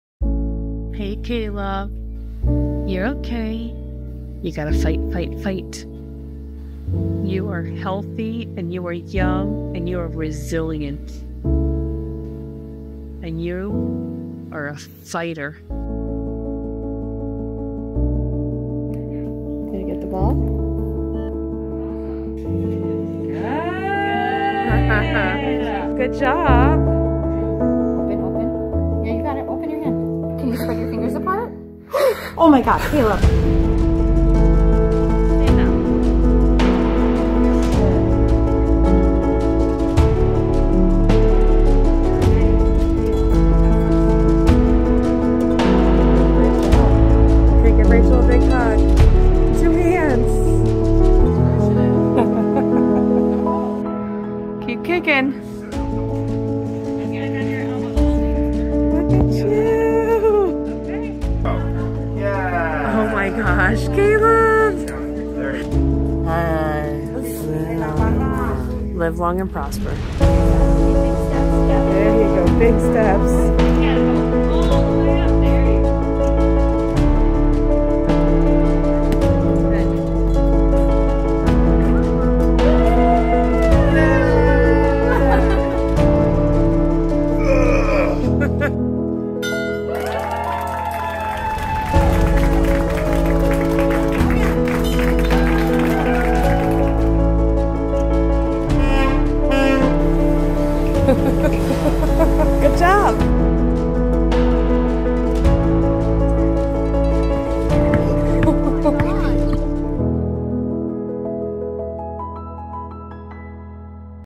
Meet Caleb: Intensive Rehabilitation After Naegleria Fowleri (Brain-eating Amoeba)
After contracting Naegleria fowleri, commonly referred to as a brain-eating amoeba, Caleb Z.’s family was told that his condition was 99% fatal. However, not only did the Florida teenager survive but, in the time since, he’s been on an intensive rehabilitation journey to relearn all skills, from walking to talking. Today, he’s using his voice and experience to educate people about Naegleria fowleri.

Watch Caleb’s grit and commitment, and learn how we care for our pediatric patients